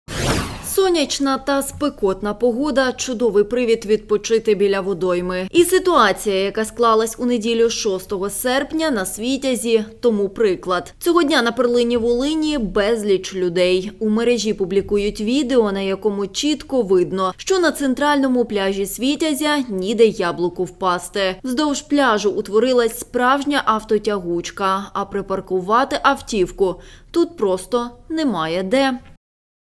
Ніде яблуку впасти😱😱 на Світязі ДУЖЕ БАГАТО людей, припаркувати авто НЕМАЄ ДЕ
Сонячна та спекотна погода чудовий привід відпочити біля водойми. І ситуація, яка склалась у неділю 6 серпня на Світязі гарний тому приклад.